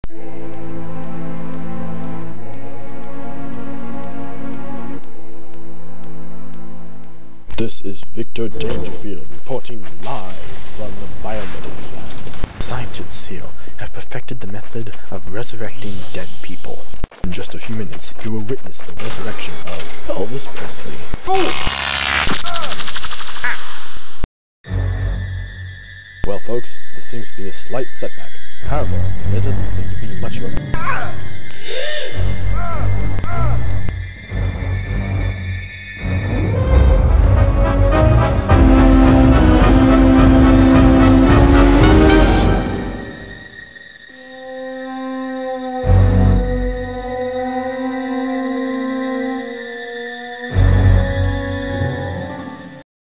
One Brick Studios Vault: A Matter of Life and Death (LEGO Studios Animation)
"A Matter of Life and Death" was one of my earliest brickfilms, made way back in 2003. I created it using LEGO Studios on my old Windows ME computer. As you can see, I didn't even have a baseplate to animate on. The poor image quality is a result of both the cheap webcam that came with LEGO Studios, and the extreme video compression LEGO Studios applied to all its films.